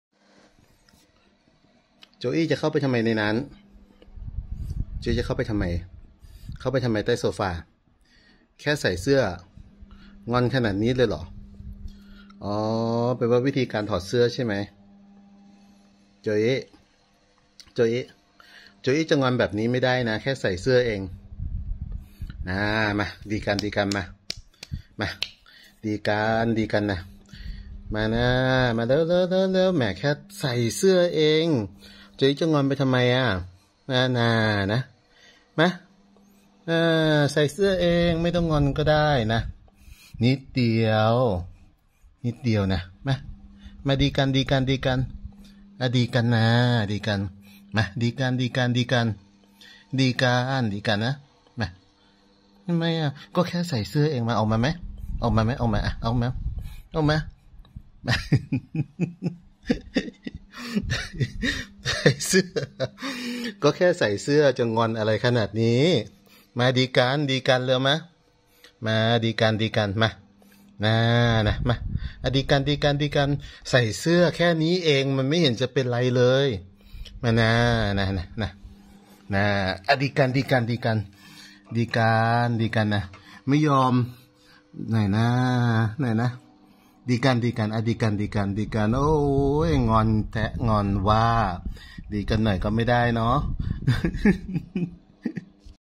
แค่ใส่เสื้อให้ ทำไมโจอี้งอนหนักมาก
แค่ใส่เสื้อให้ โจอี้ หมาน้อยอายุเดือนกว่าๆถึงกับงอนหนักมาก